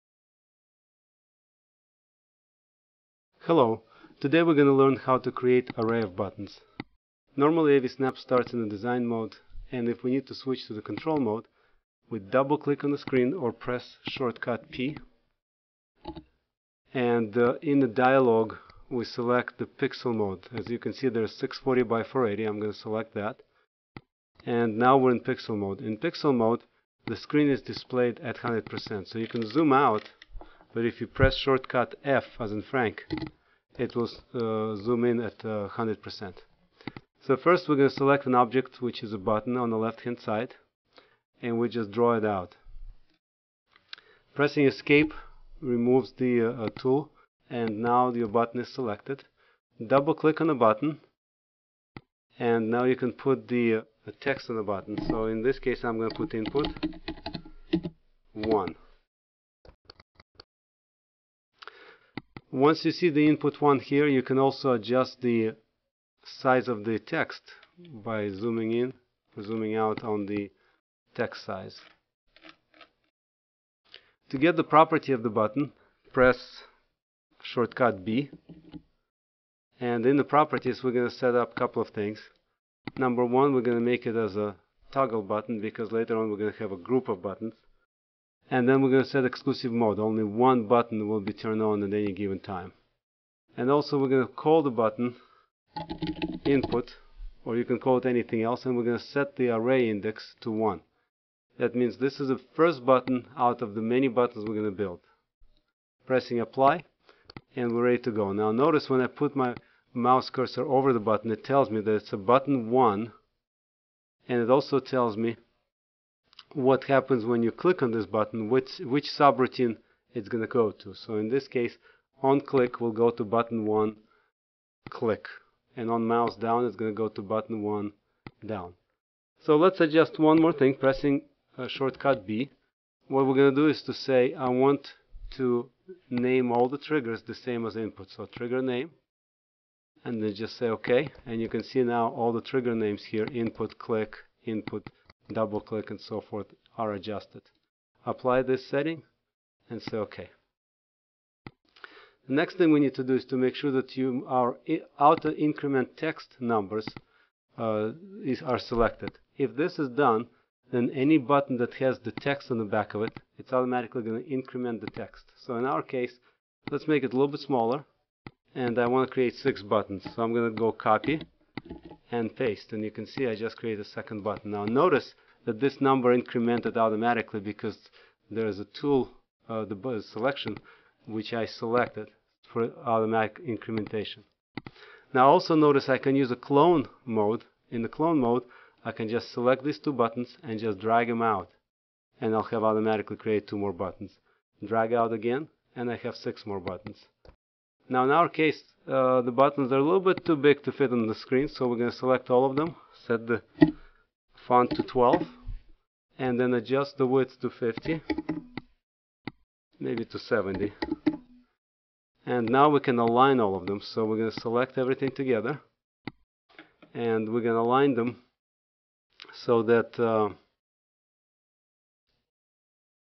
Creating array of buttons.avi
This tutorial explains how to create array of buttons in AVsnap. Array of buttons is useful when creating GUI for controlling switchers, DVRs and Cable boxes.